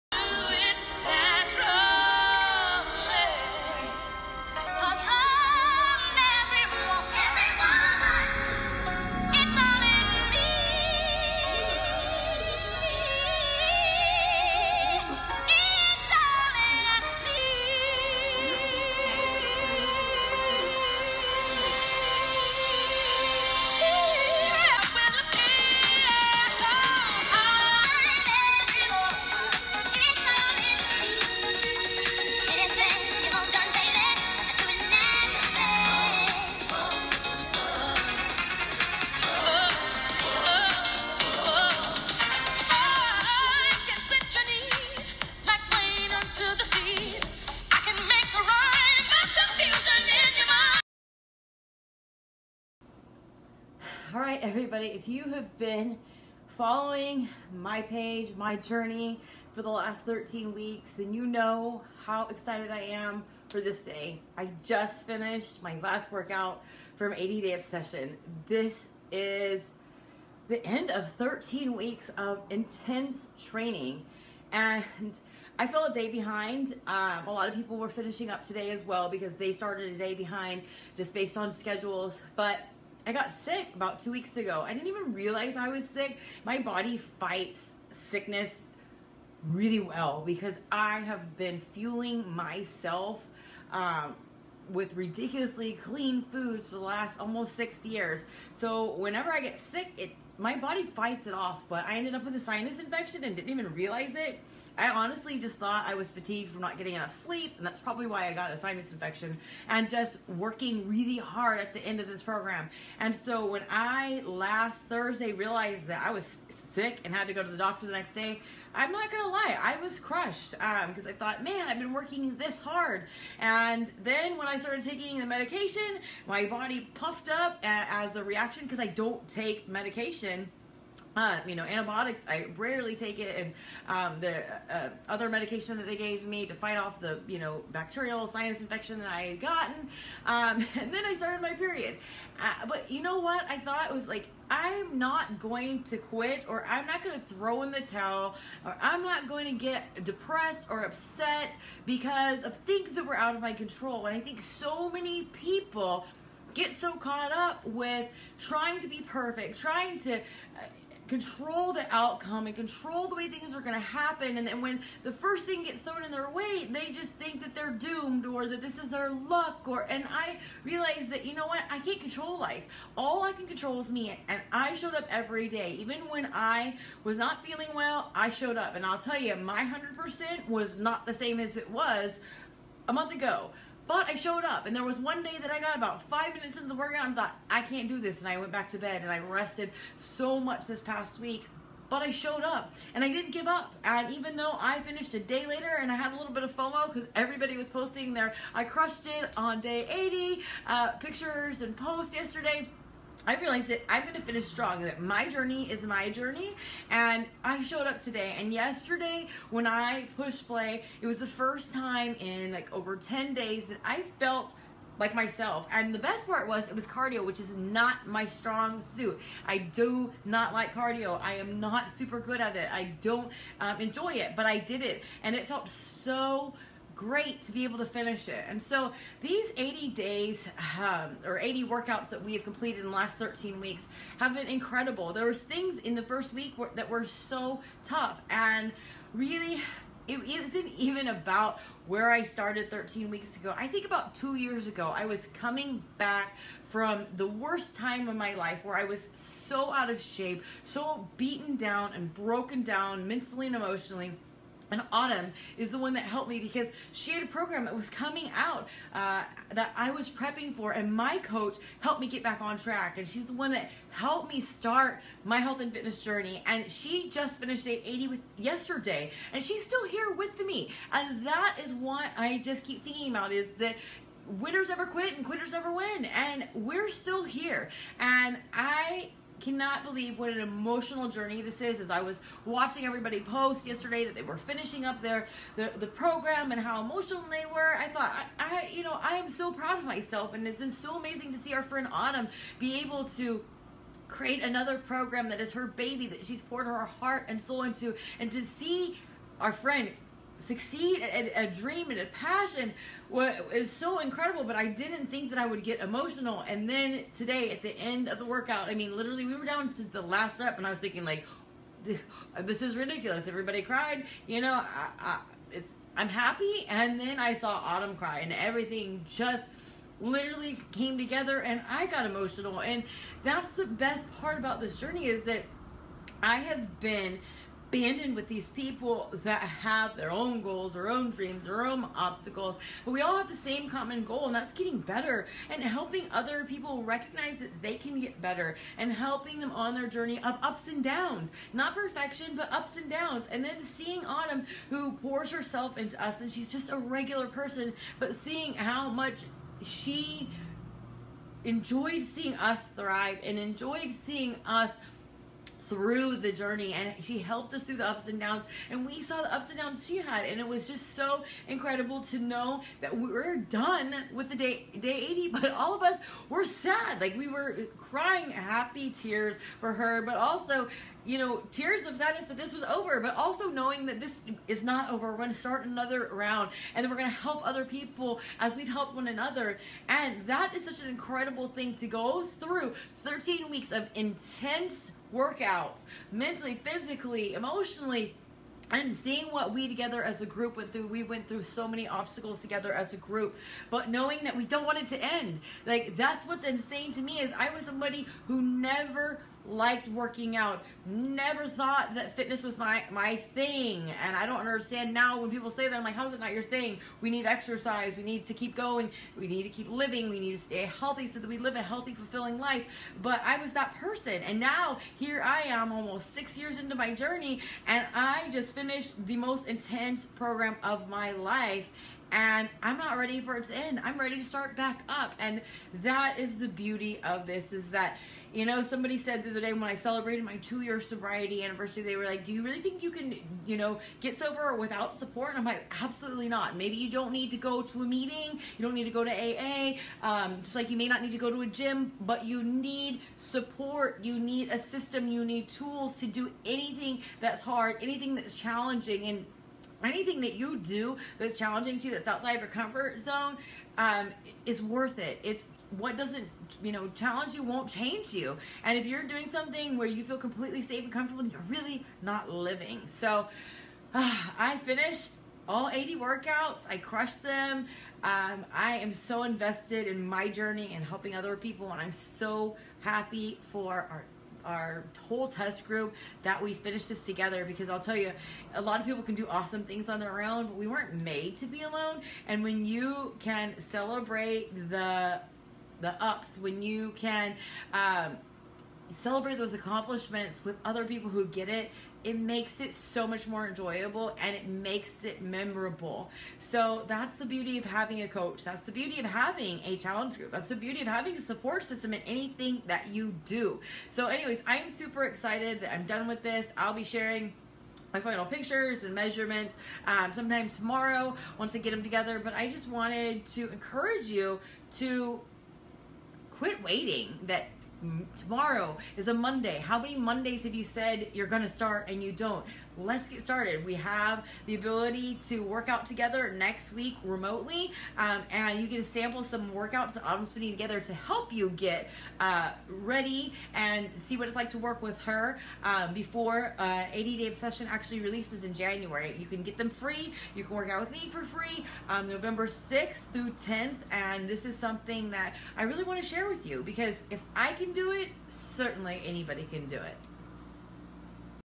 Day 80 of 80 Day Obsession - My Final Thoughts
Our CEO 👨‍💼 Carl suggested we document the moment we completed 80 Day Obsession
Due to unforeseen circumstances I ended it a day later than anticipated. Yesterday I saw my fitness buddies posting that they finished it and many talked about how emotional it was. I totally didn’t think I’d react the same way. I was more than proud of myself today as I was completing each move, each rep, each set, but I didn’t think I’d cry. But then Autumn started talking about how her son Dom has started to voluntarily adopt a love for working out and even ate oatmeal and broccoli for breakfast the other day and I looked over at Rebel who was sitting at my desk and had stopped to listen to what she was saying. I started to cry because I’ve watched her son Dom grow up via Social Media just as many have watched my son Rebel and even my daughter Emily closely follows Autumn and Doms life via Instagram. I started to really become even more grateful for what I’m part of. It’s not just about me looking good or feeling good. It’s about making me a better person of influence to be the role model my children and your children deserve. And as the last rep was over and Autumn started crying, I lost it. I was feeling all the feels. I’m so proud of me. So proud of Autumn. So proud of our group. So proud of my coach Becky and my many friends including Kristina & Monica and so many others for finishing S T R O N G 🍑🙌🏽.